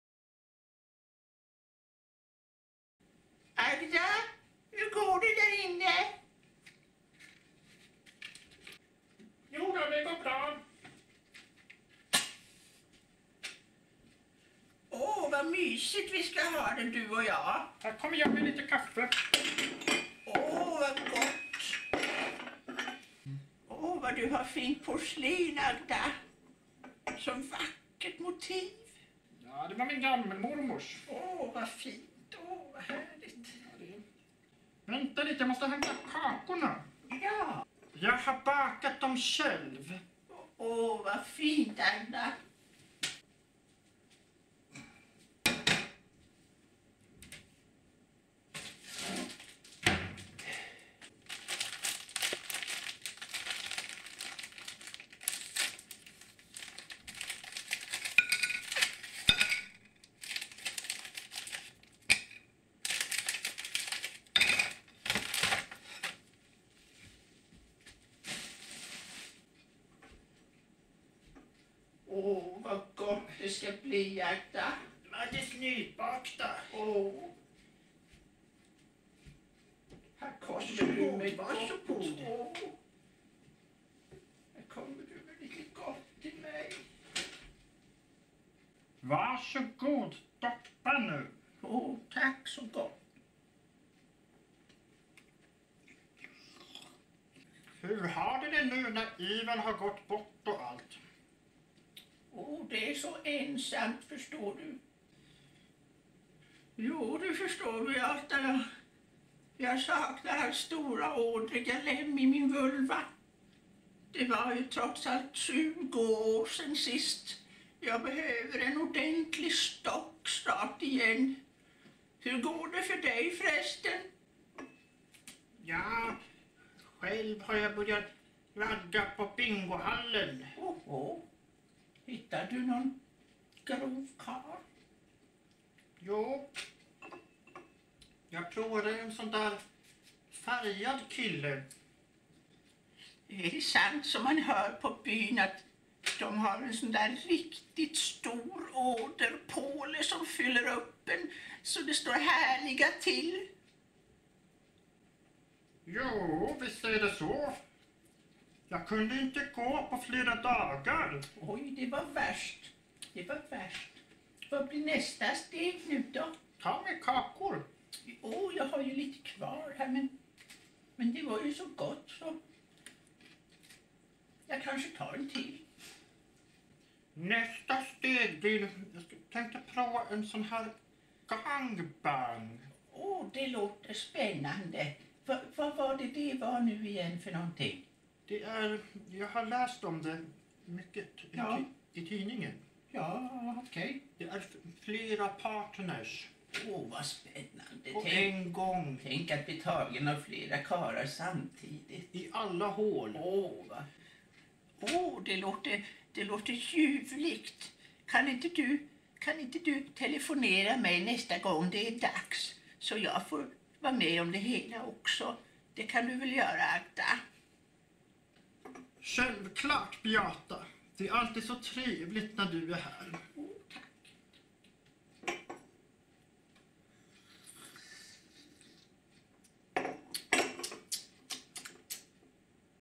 Agda & Beata - kafferepet
Agda och Beata ska ha kafferep och diskutera djupa ämnen i livet.